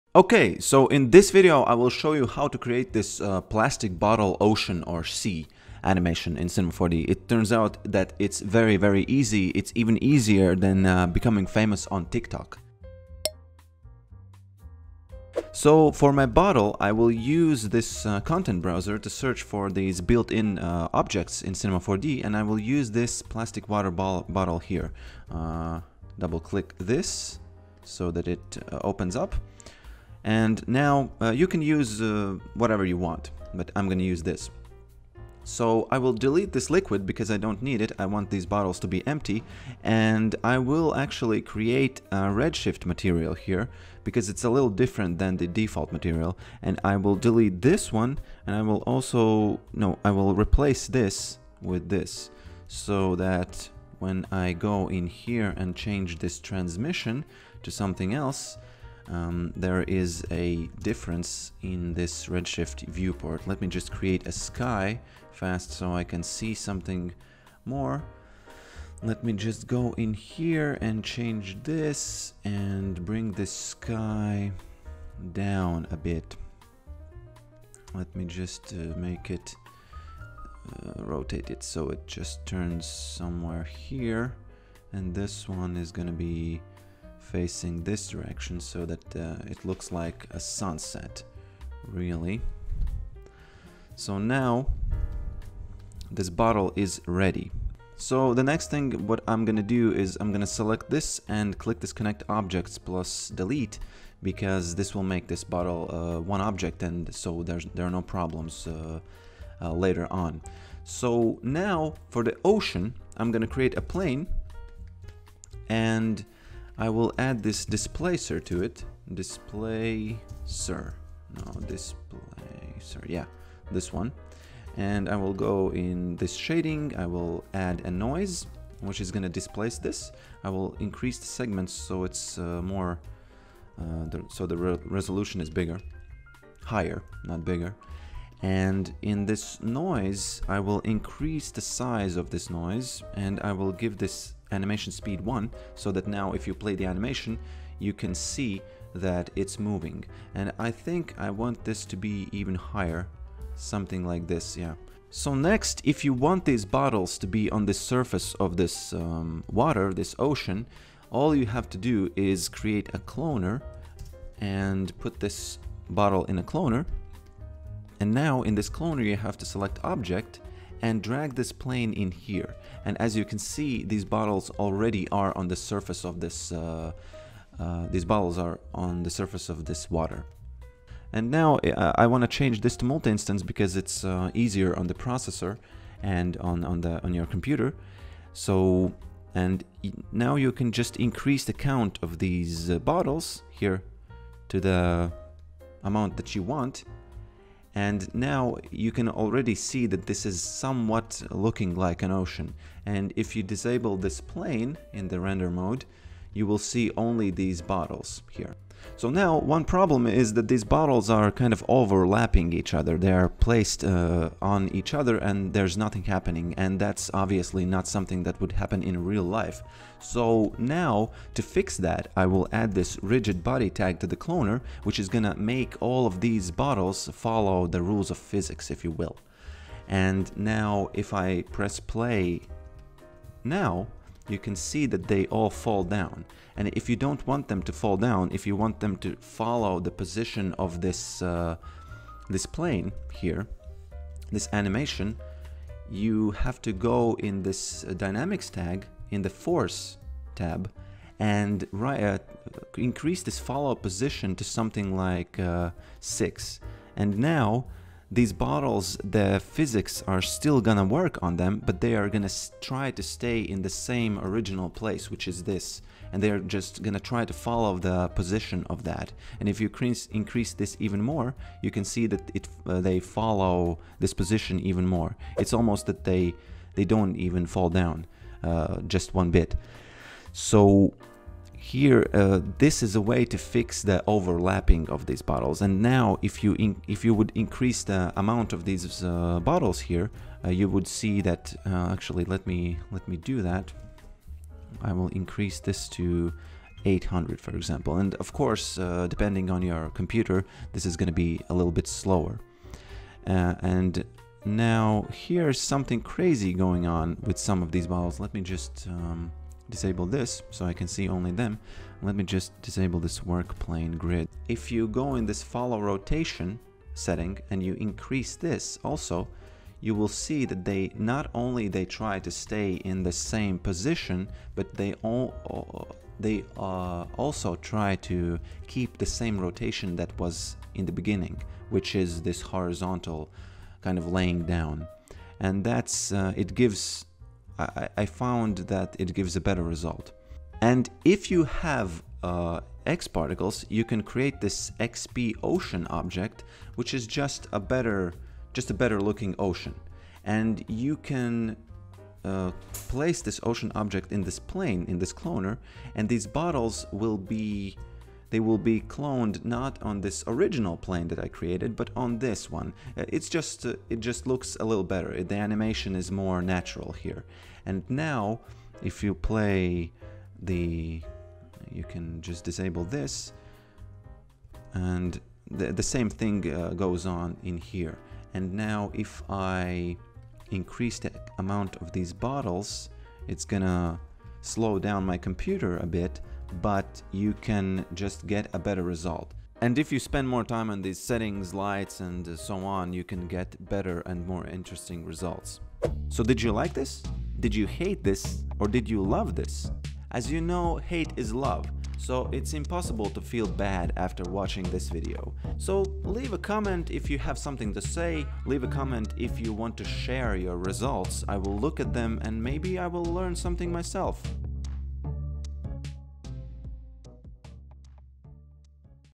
Cinema4D Tutorial: Sea of Bottles
How to create dynamic bottles in a sea using c4d, Redshift.